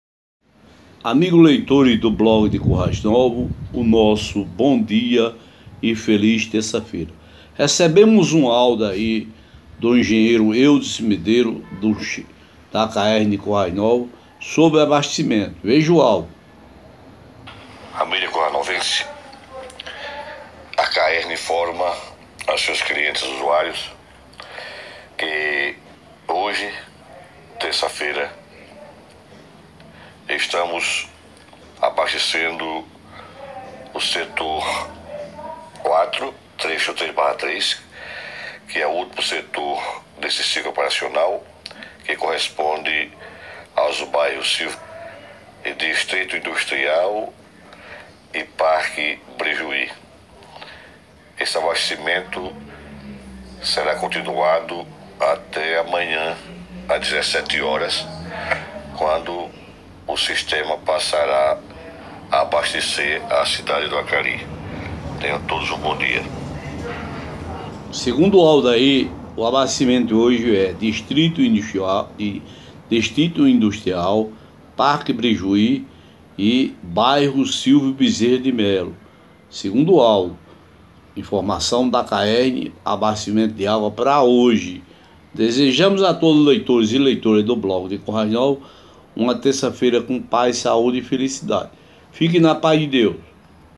Notícia do abastecimento de água de Currais Novos, para essa terça feira 24 de julho de 2018.
Vídeo de Inacio de Colombita